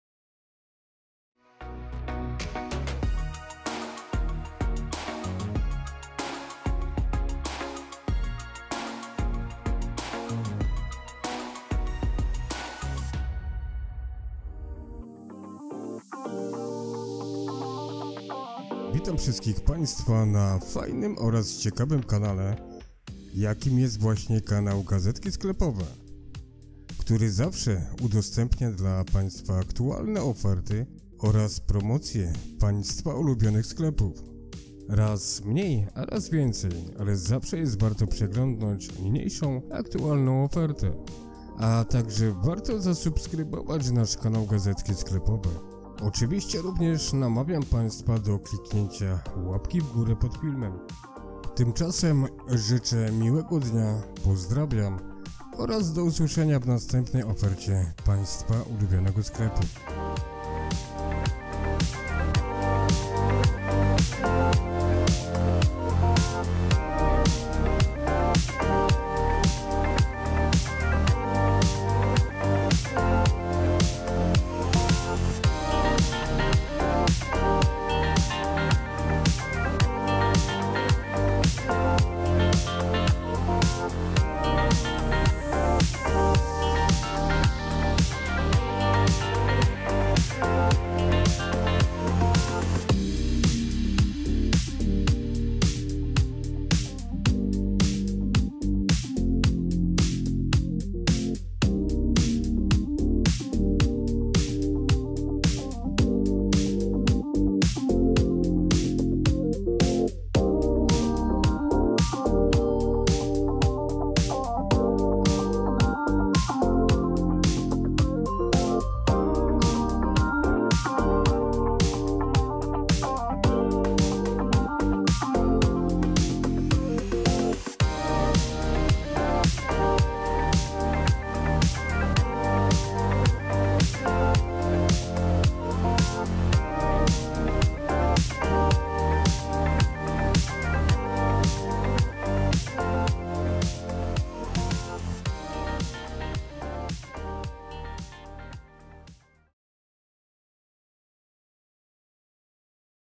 Lidl | Nowa Oferta Promocyjna Od 27.04.2023 | Wiosna w Twojej Kuchni – Kuchnia Dobrych Rozwiązań
Już od Czwartku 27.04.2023 Nowa oferta Lidla | Co Nowego w Kuchni
    • Blender z funkcją gotowania
    • Kuchenka mikrofalowa
    • Spieniacz do mleka, z uchwytem
✓ Perfekcyjnie spienione mleko w krótkim czasie
✓ Łatwa obsługa przez naciśnięcie przycisku
✓ Uchwyt do łatwego przechowywania
    • Akcesoria kuchenne
i wiele innych ciekawych propozycji w dobrych cenach.
A Co znalazło się tym razem z pewnością dowiemy się oglądając niniejszy film.
A więc zapraszam na film i na udane zakupy do Lidla
Pobierz aplikację Lidl Plus i kupuj jeszcze taniej